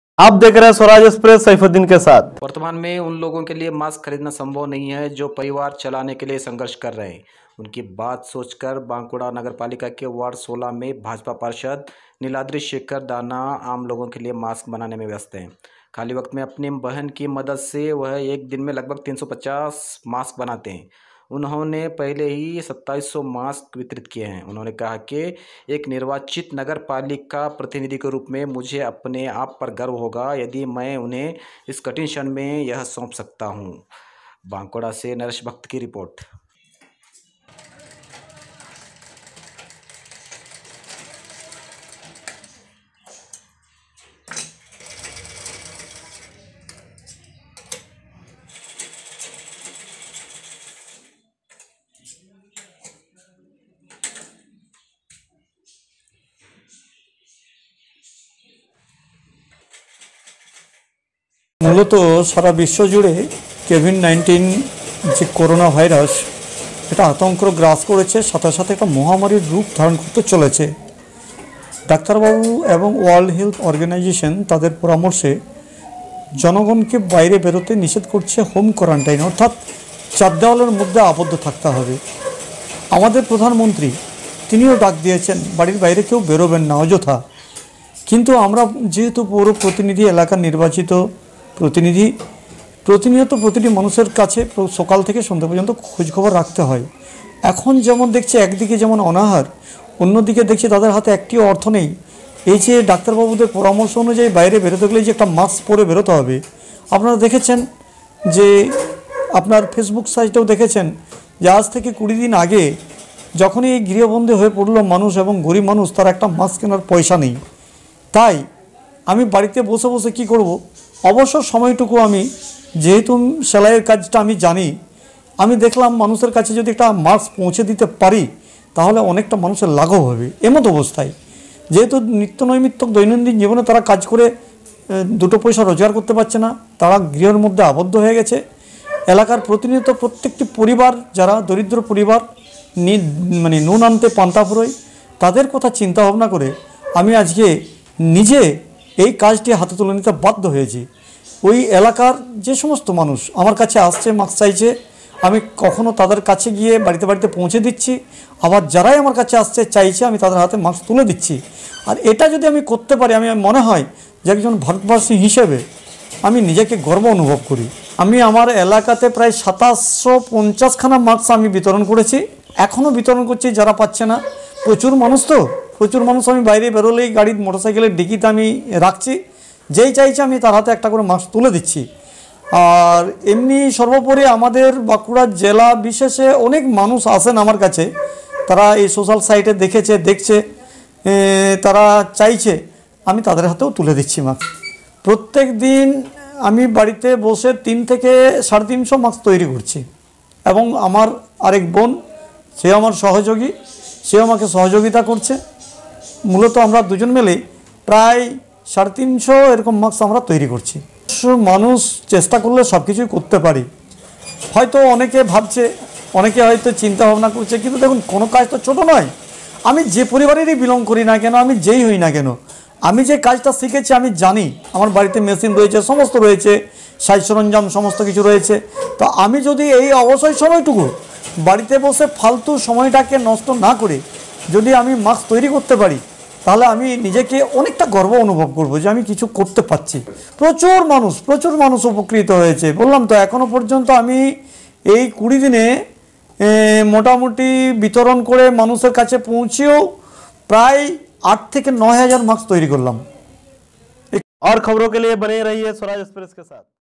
भाजपा पार्षद निलाद्री शेखर दाना आम लोगों के लिए मास्क वनाने में व्यस्त हैं। खाली वक्त में अपनी बहन क
भाजपा पार्षद निलाद्री शेखर दाना आम लोगों के लिए मास्क वनाने में व्यस्त हैं। खाली वक्त में अपनी बहन की मदद से, वह एक दिन में लगभग 350 मास्क बनाते है। उन्होंने पहले ही 2700 मास्क वितरित किए हैं।